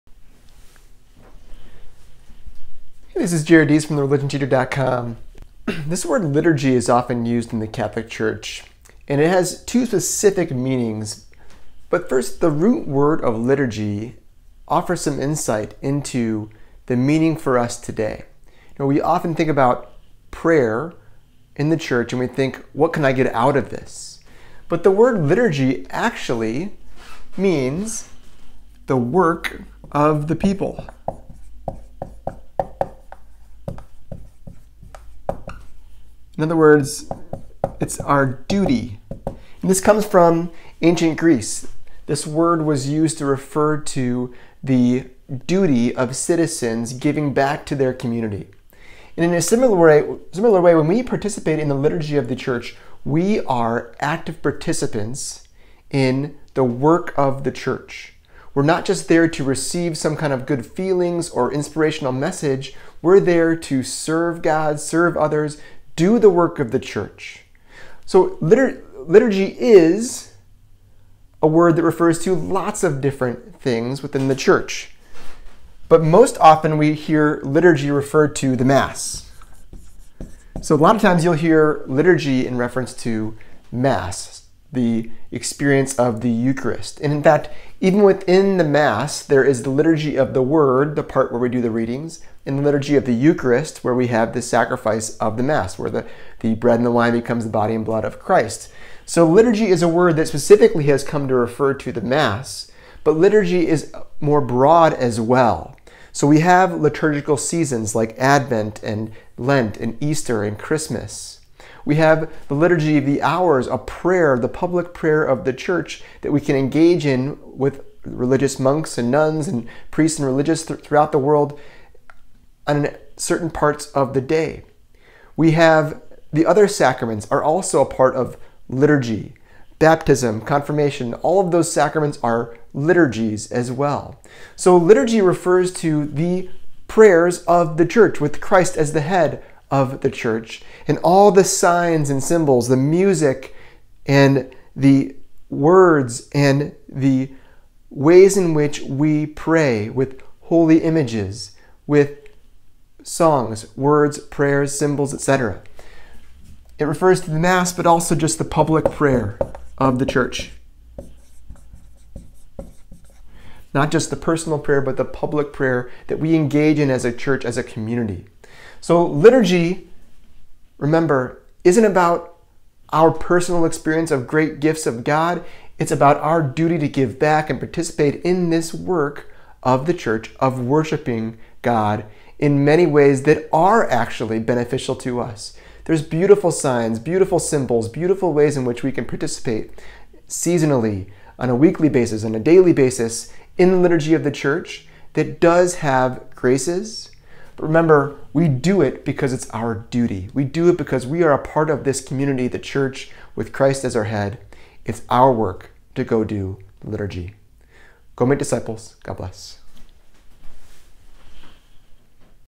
The Meaning of Liturgy in the Catholic Church
What does liturgy mean in the Catholic Church?

The origin of the word liturgy is the Greek word "leitourgia," which means "work for the people." In ancient Greece, the citizens would do public service for their city. They would give back to their community. It was their duty as citizens.

leos/laos = people, public
ergo = to do

In the Catholic Church, we use the word liturgy to refer to the public prayers of the Church, especially the Mass. It includes the rites and rituals that we do together as a Church to worship God.

The Liturgy of the Hours, for example, is the public prayer of the Church that all religious and even laity can join in together at specific times of day.

We have come to use the word liturgy to especially refer to the Mass. Even within the Mass there are two parts that particularly bear this name: the Liturgy of the Word and the Liturgy of the Eucharist.

But let's go deeper. What is actually happening during Liturgy?

WHO?

Liturgy is an "action" of the whole Christ (Christus totus). Those who even now celebrate it without signs are already in the heavenly liturgy, where celebration is wholly communion and feast. (CCC, 1136)

HOW?

- Signs and Symbols
- Words and Actions
- Singing and Music
- Holy Images

WHEN?

- The Mass
- Liturgical Year, Seasons
- Liturgy of the Hours

WHERE?

- Anywhere, but especially a church with all the signs that help elevate our minds and hearts to God.